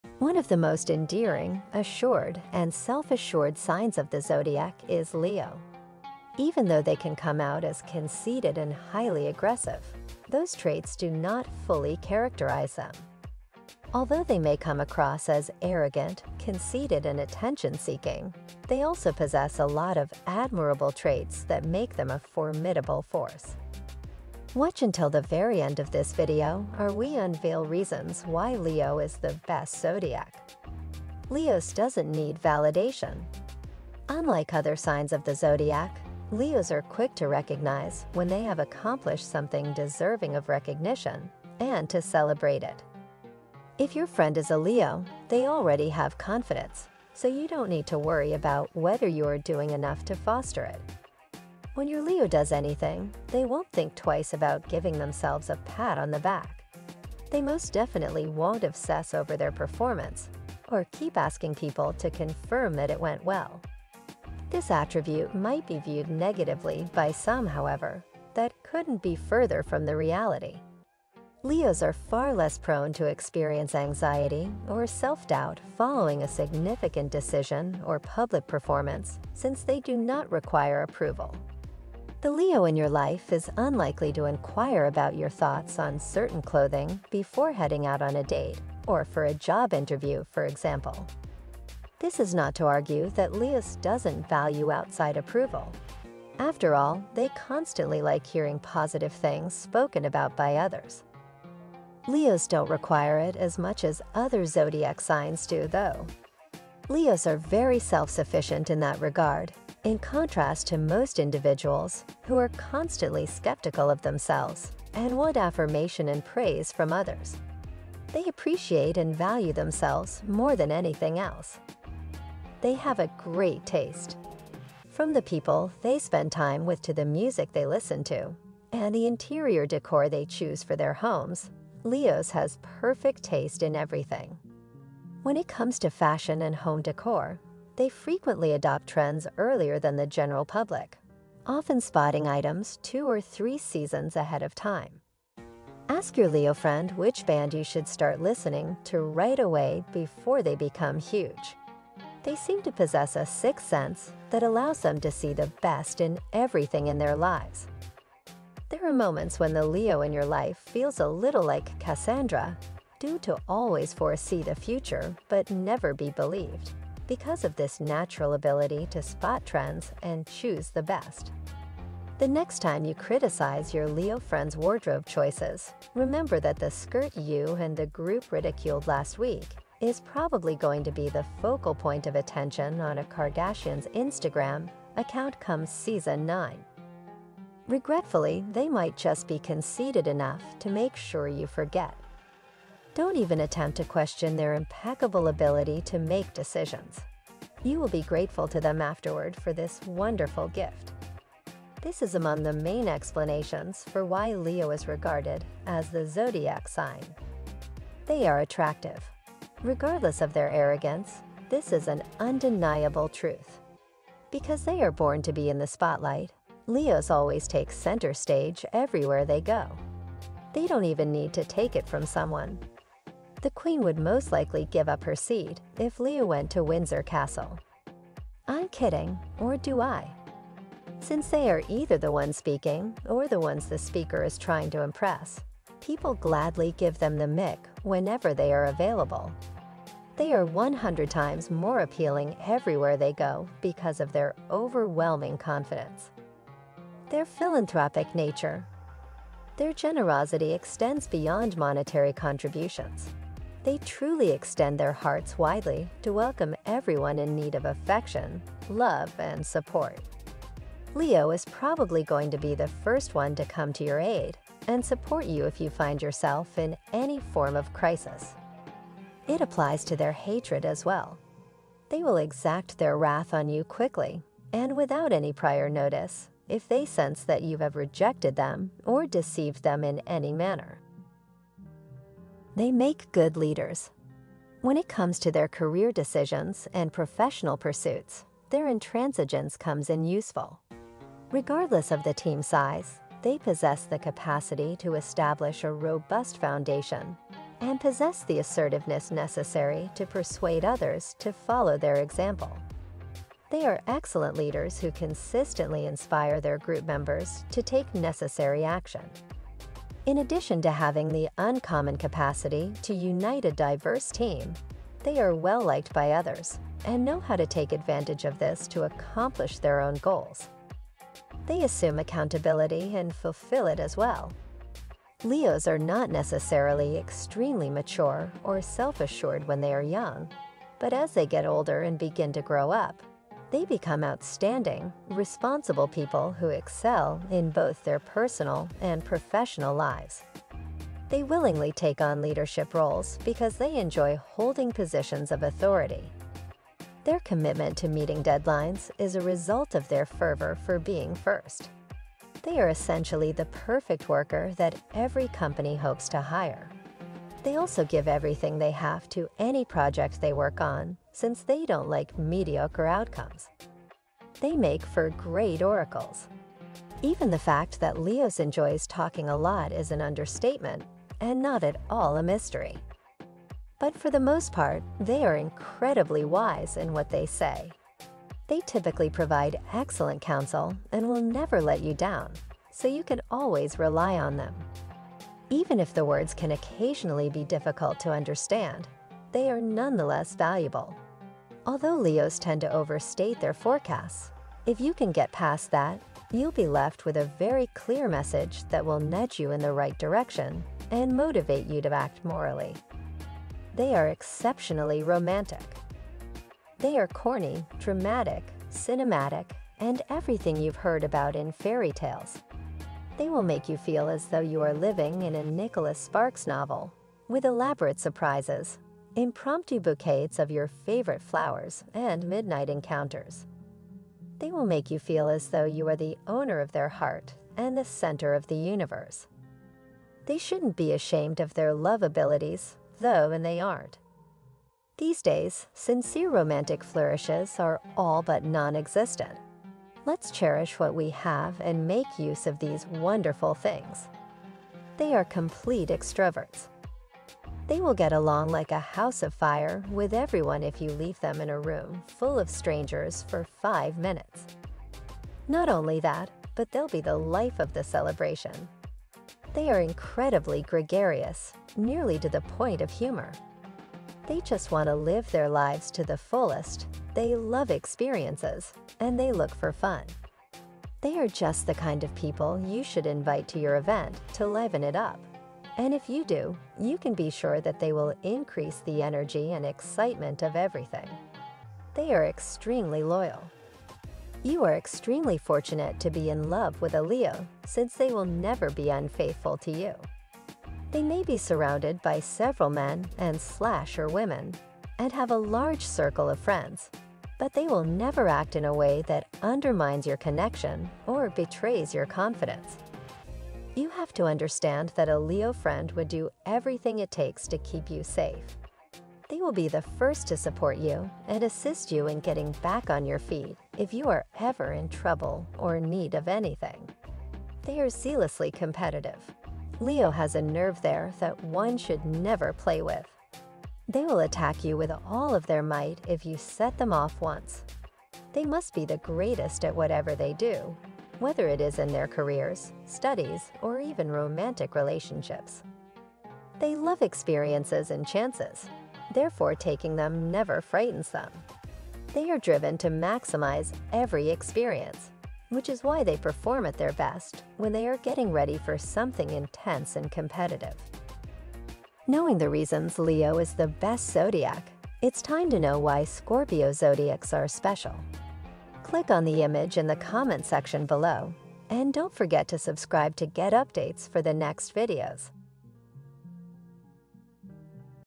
Reasons Why Leo Is The Best Zodiac Sign
One of the most endearing, assured, and self-assured signs of the zodiac is Leo. Even though they can come out as conceited and highly aggressive, those traits do not fully characterize them. Although they may come across as arrogant, conceited, and attention-seeking, they also possess a lot of admirable traits that make them a formidable force.

🐲 Thank you very much for Your support. Only because of YOU we can make these awesome videos! 🙏